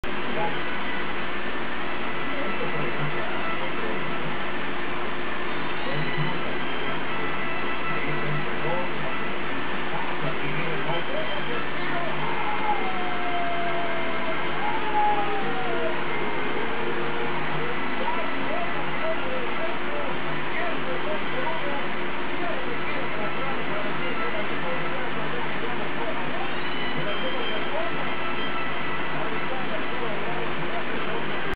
Dorados vs Veracruz Gol de Cuauhtémoc Blanco
Gol de Cuahutemoc Blanco que dá el triunfo a Dorados 2 - 1 sobre Veracruz en la jornada 1 de la Liga de Ascenso MX. Culiacán, Sinaloa. 21/07/2012